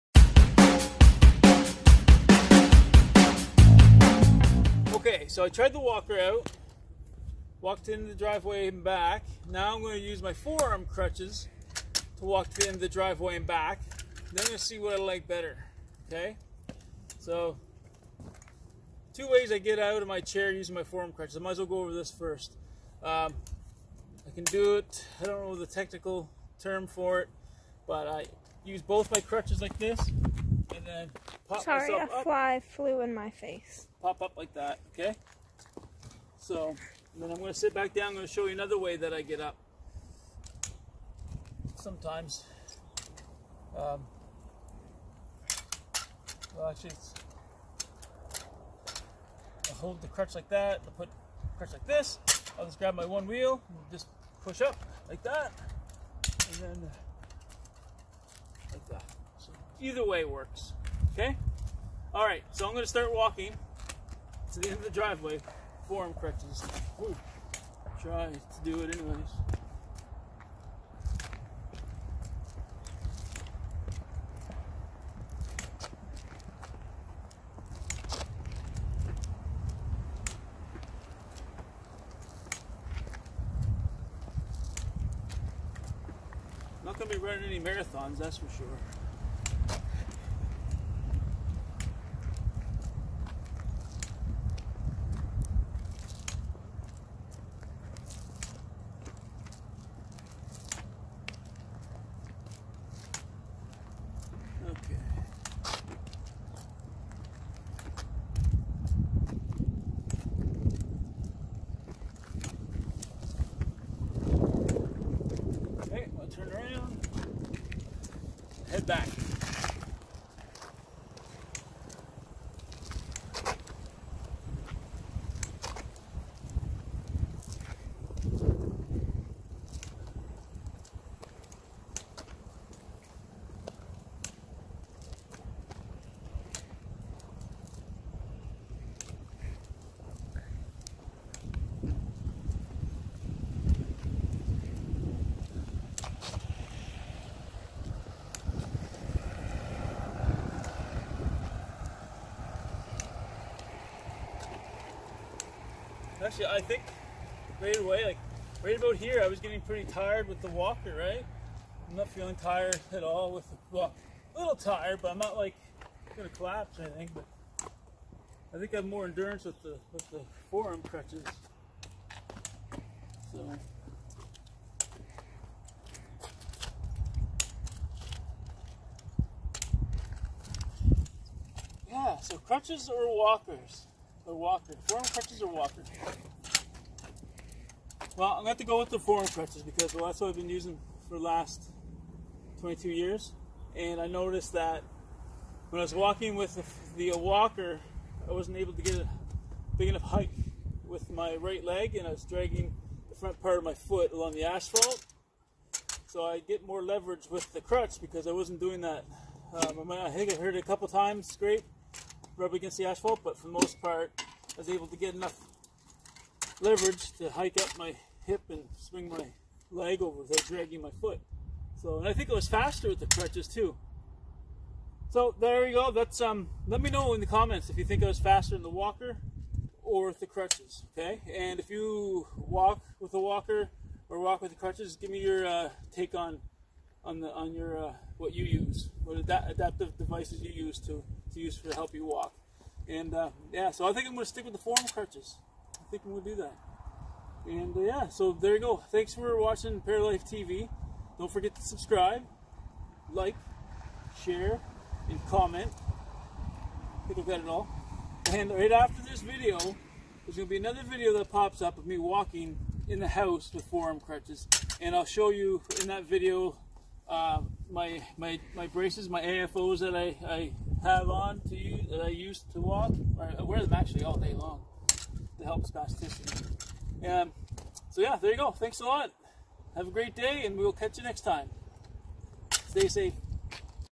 Paraplegic Walking - Walker vs Crutches Part2!
Here it is, Part 2 of WALKER vs CRUTCHES!

Well after my walk with the forearm crutches I must say I need to walk more often! I am out of shape when it comes to walking. SO the answer, I definitely prefer to walk with the forearm crutches. Big reason being it's what I'm used to. I've been using forearm crutches for 22 years now and feel very comfortable using them. I found with the walker I was more stable holding onto the walker but I could not get enough leverage to "hike" up my right leg so my foot wouldn't drag. Which you can hear in the video PART 1.

Also, travelling, the walker does fold down but takes up more room that the crutches. The crutches can also be adjust to a shorter length for easy storage.

So there you have it! I'm sticking with the forearm crutches!